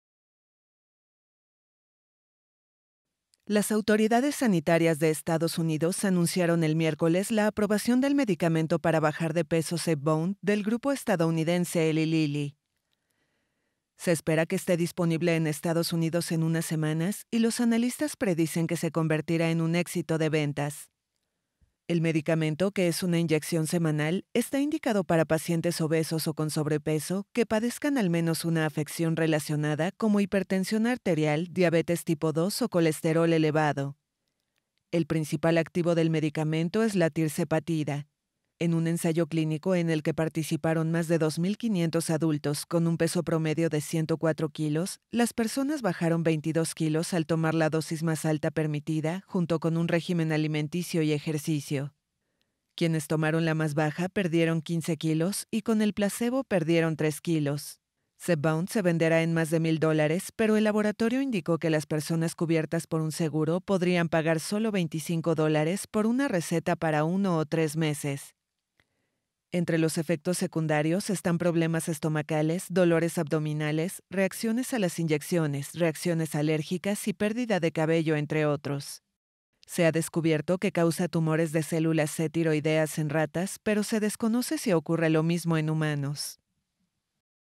EEUU aprueba esperado fármaco de Eli Lilly para pérdida de peso
Las autoridades sanitarias de Estados Unidos anunciaron el miércoles la aprobación del medicamento para bajar de peso Zepbound, del grupo estadounidense Eli Lilly, la última incorporación en el campo de los nuevos y poderosos fármacos para la obesidad.